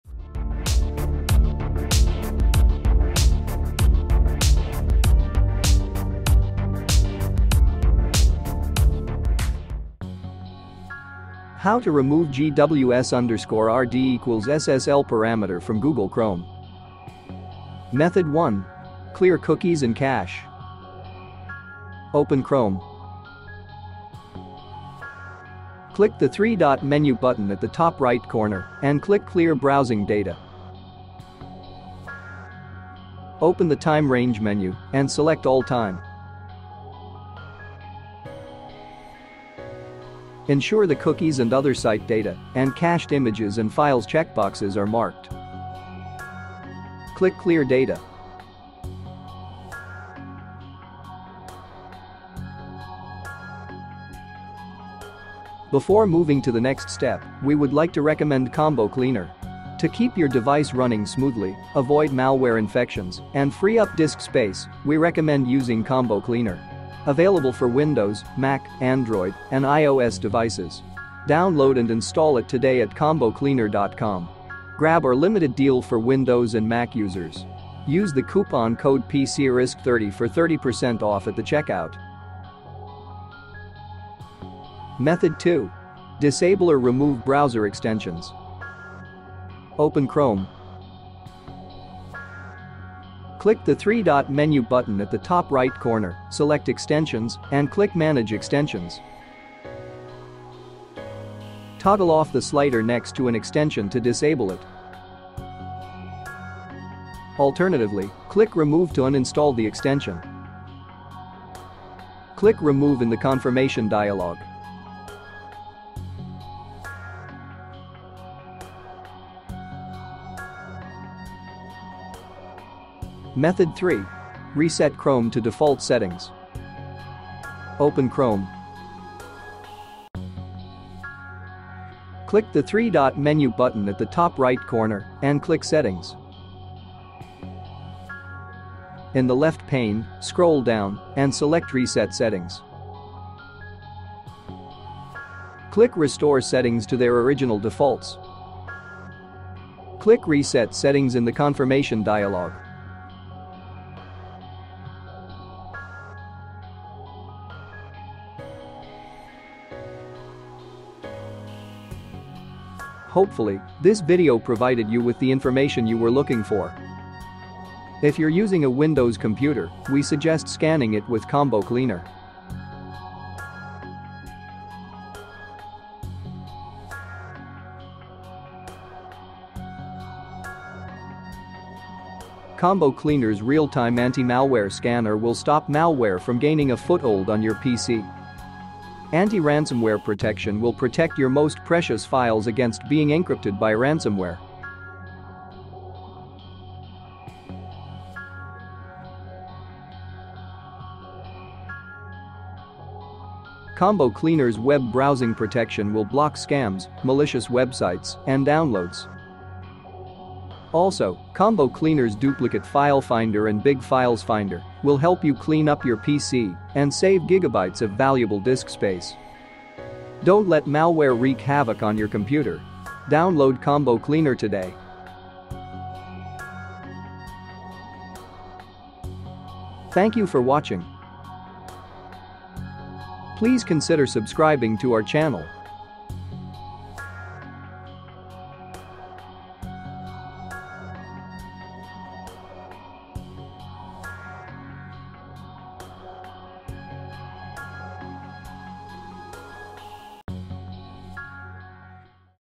How to Remove gws_rd=ssl Parameter from Google Chrome.
Video showing how to remove gws_rd=ssl Parameter from Google Chrome.